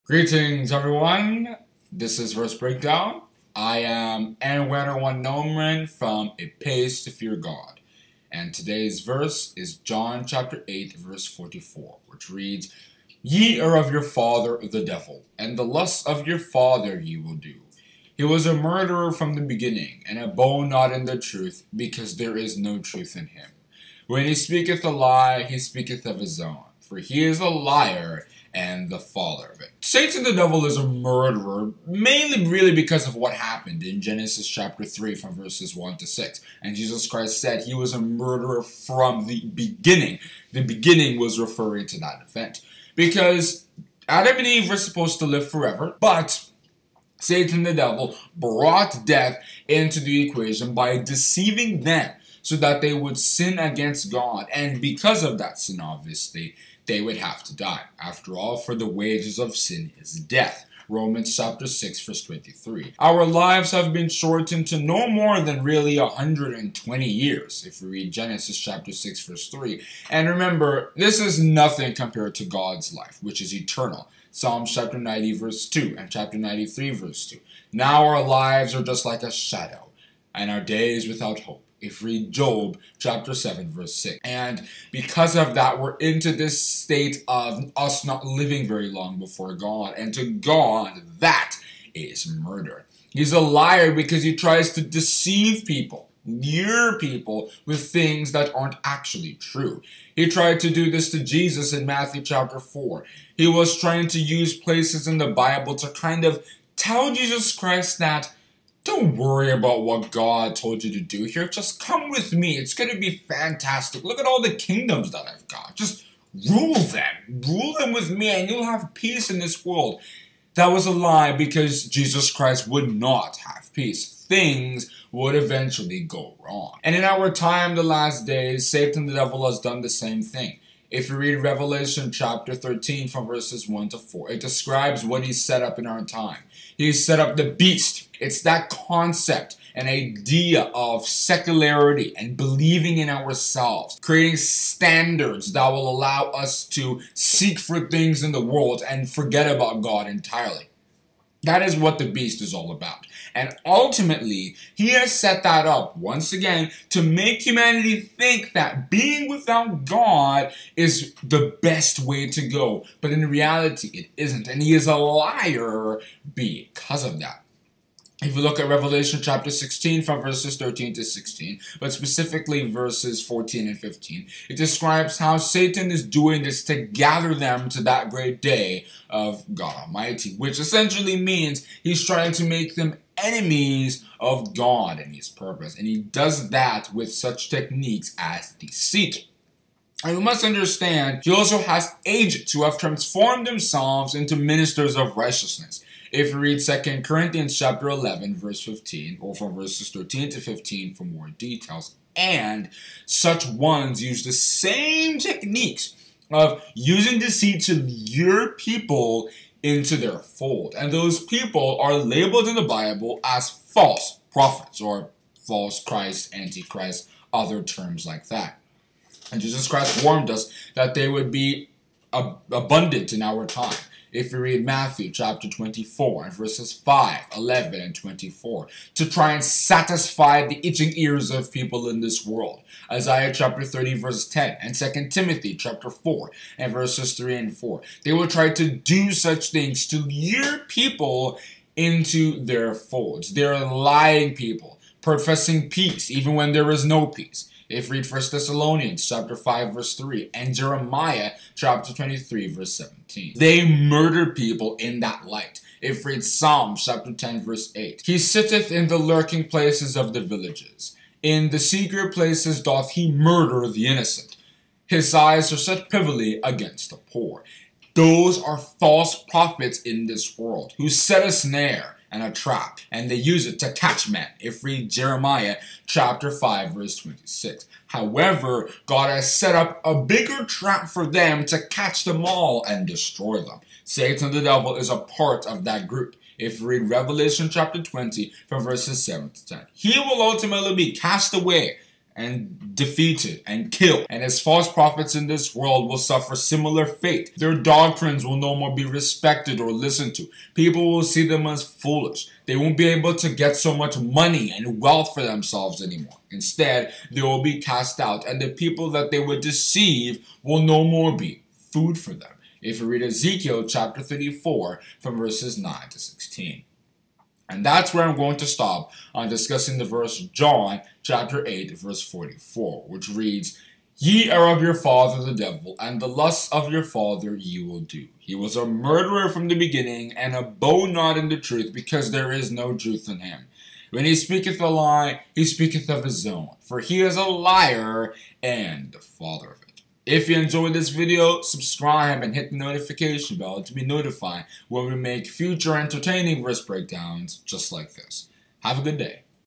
John 8:44 - Verse Breakdown #70 | Ewaenruwa Nomaren
Jesus Christ made a very offensive statement to the Pharisees in John 8:44, by associating them with Satan the Devil.

But at the same time, it does give some good information about who Satan the Devil is. And in this short video, we will examine Satan the Devil using that statement, and pick examples of things he's done and doing that fulfill Jesus Christ's saying.

If you would like more information about God, His Kingdom, and His Son Jesus Christ, please check out these resources:

Our Book

Want to share your opinion?